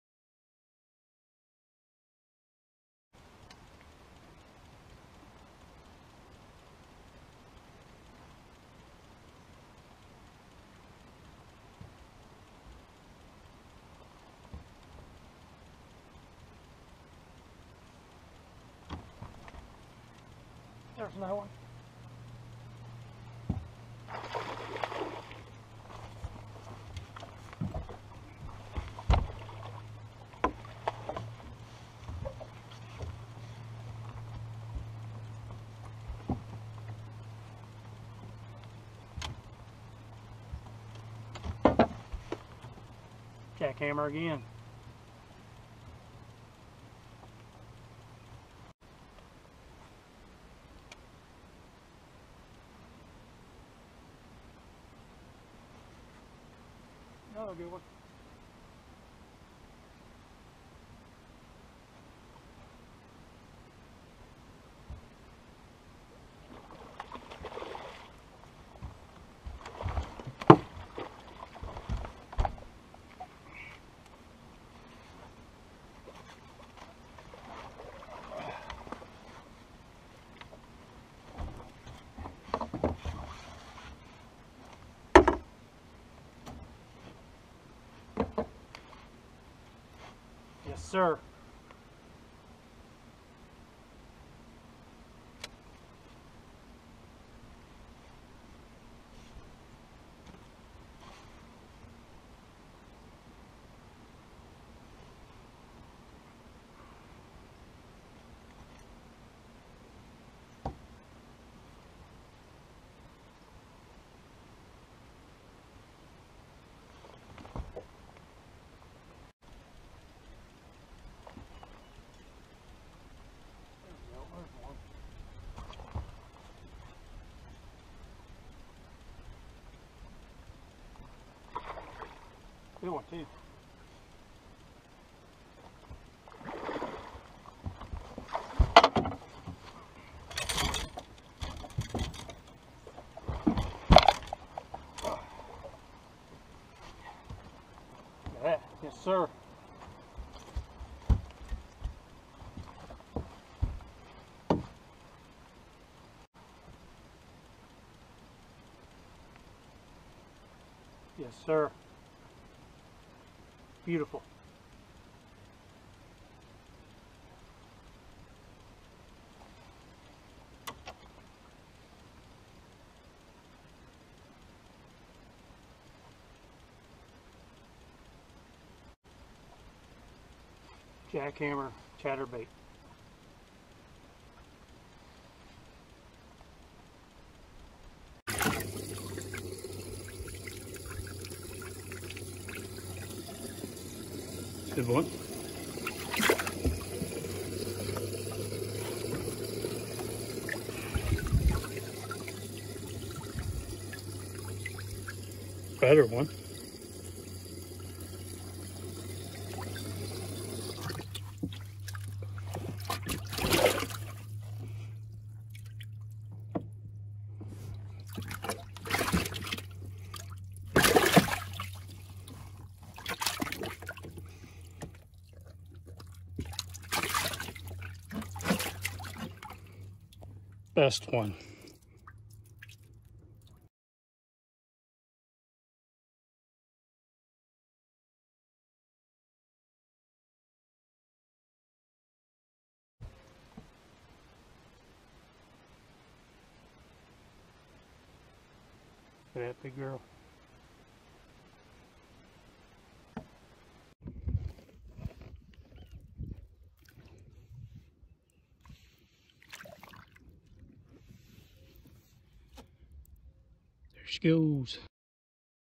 Giant pre spawn Bass day thanks to the Zman jackhammer
Zman jackhammer helping me have a great day on the lake. The pre-spawn was hot and heavy today. With murky water the chatterbait made the perfect sound to attract some big bass.